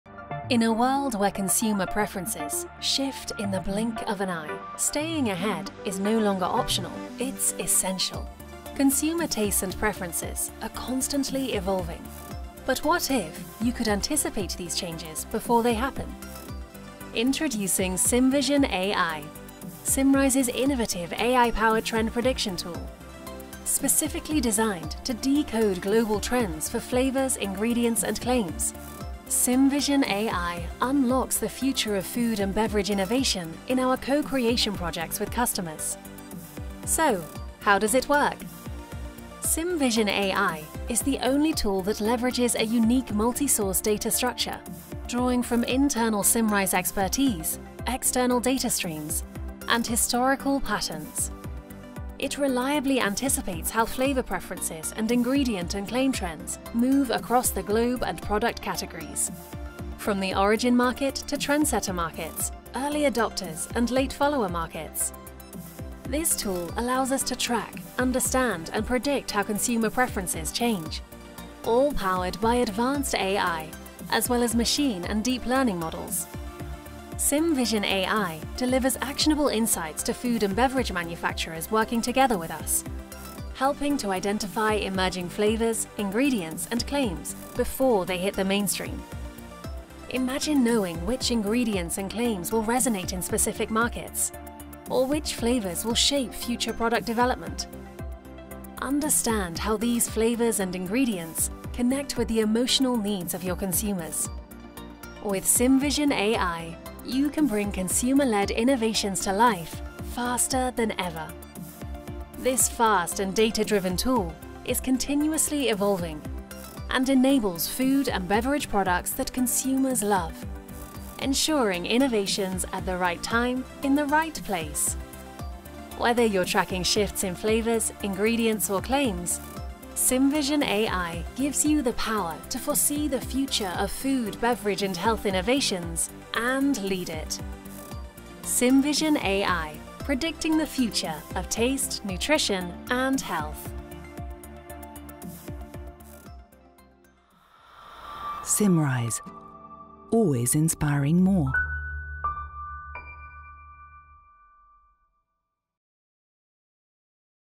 Symvision AI™
An ever-changing world makes decoding and predicting trends crucial to address consumer needs and develop market relevant food and beverage innovations.

Anticipate the future of taste, nutrition and health with Symvision AITM, our tool to predict trends for flavors, ingredients and claims based on AI and a unique data structure – reliably.